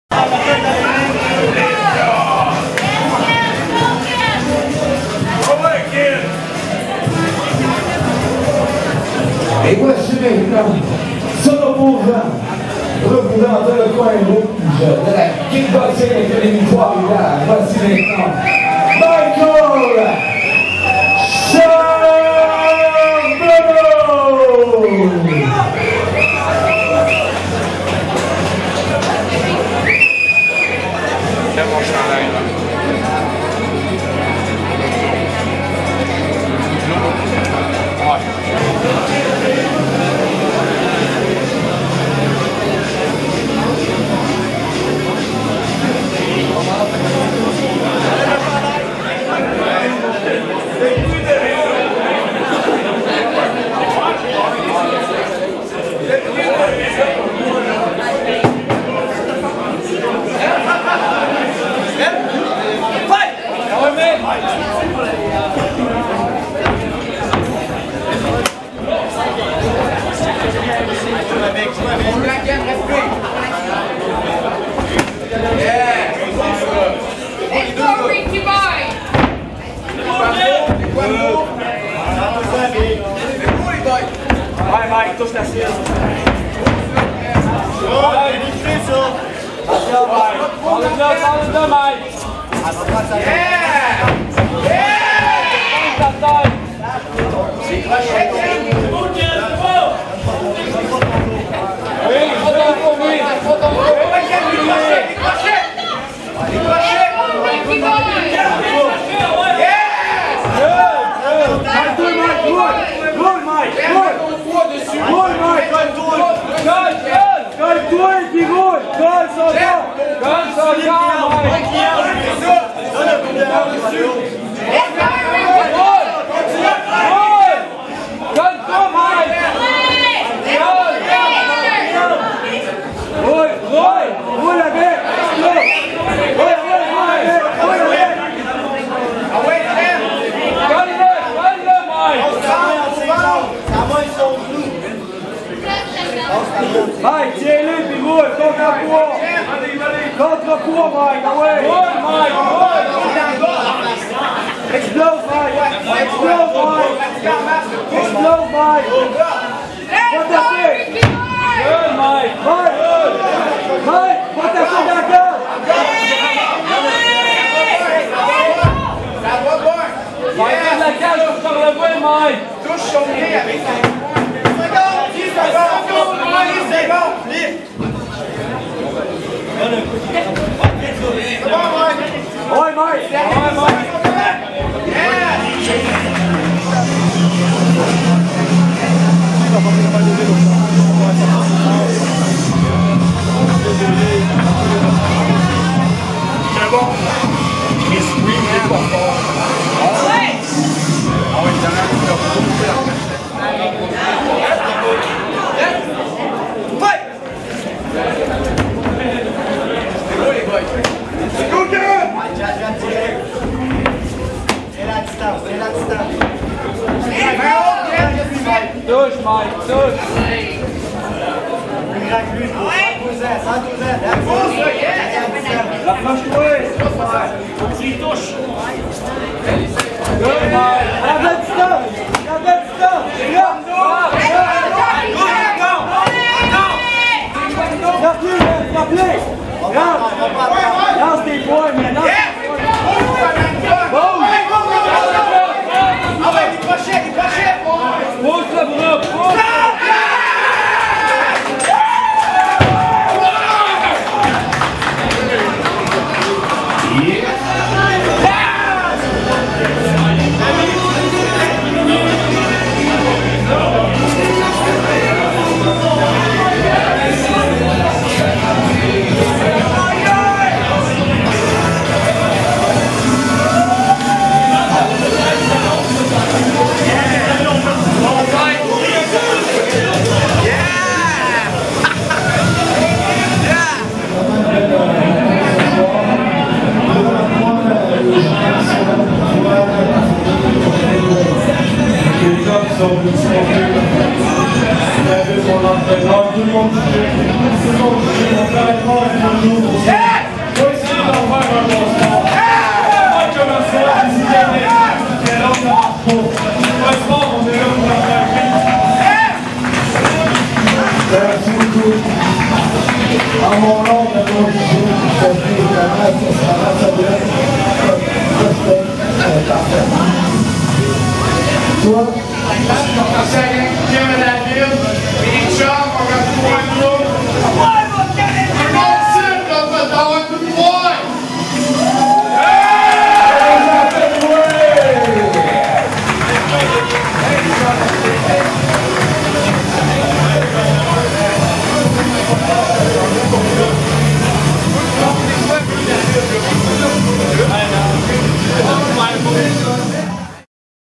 8 Ken Hubert VS Michael Charbonneau
Ken Hubert affronte Michael Charbonneau en combat dans une cage à trois Rivières, Québec, Canada le 11 février 2017.
Images gracieuseté de David Hubert Therrien